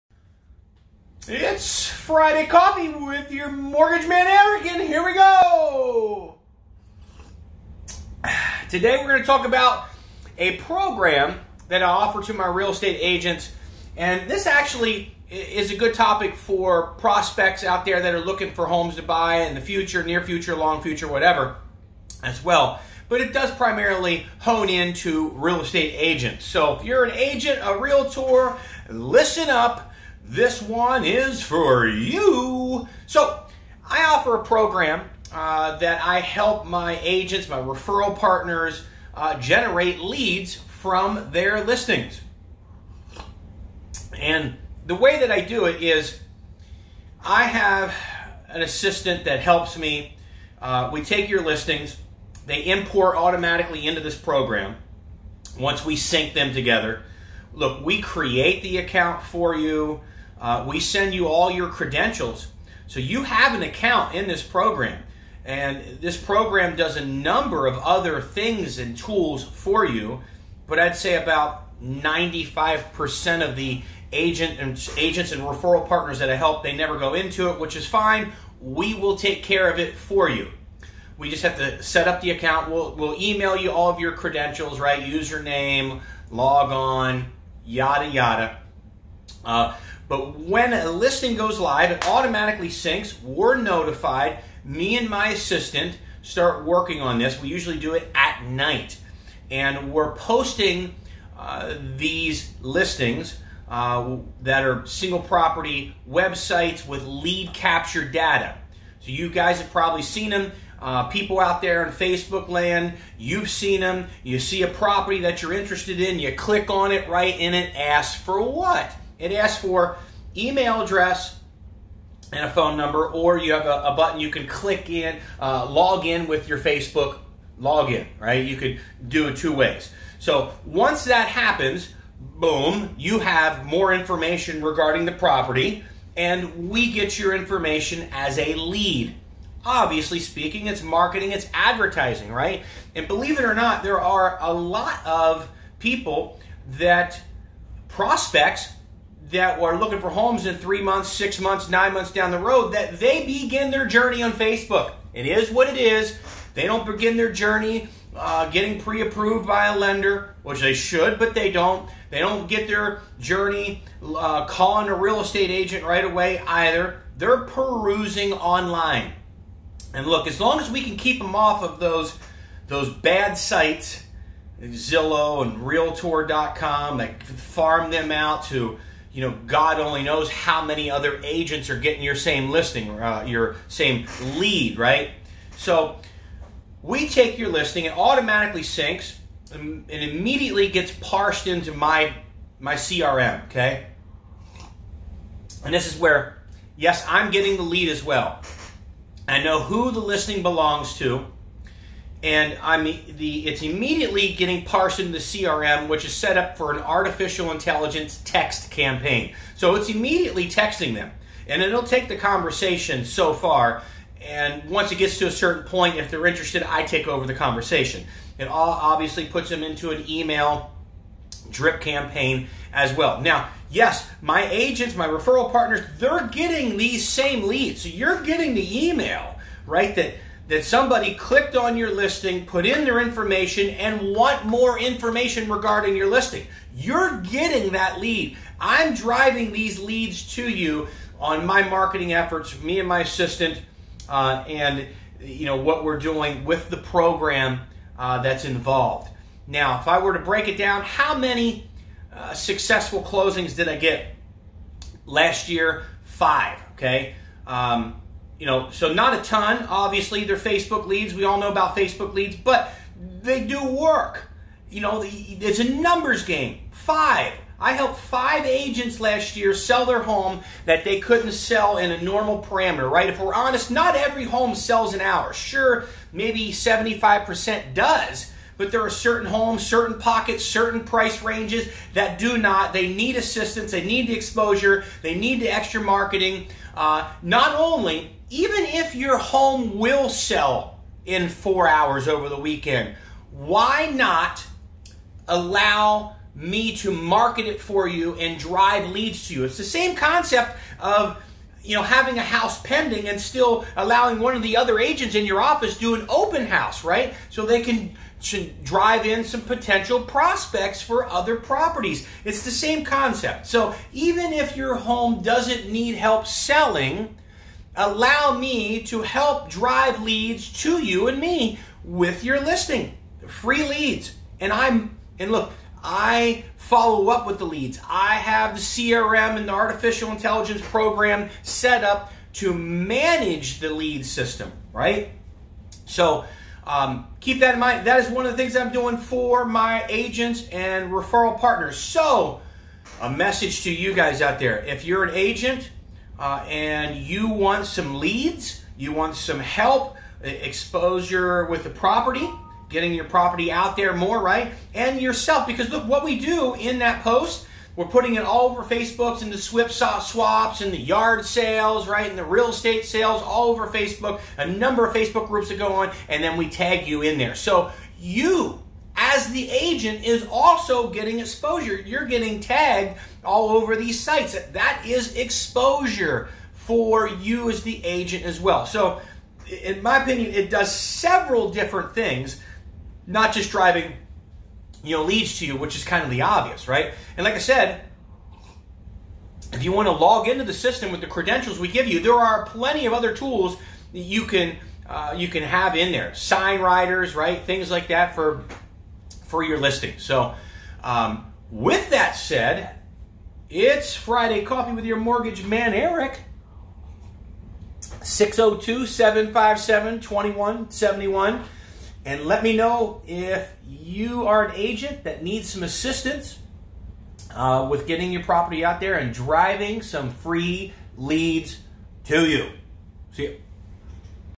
Friday Coffee with your Mortgage Man Eric
Real Estate Agents, do you want Leads?: If you are a Real Estate Agent and you are wanting some Leads, please watch this video. I can send you Leads. I can get more exposure for your Property and more exposure for YOU as well. All for Free! Tune in, here we go...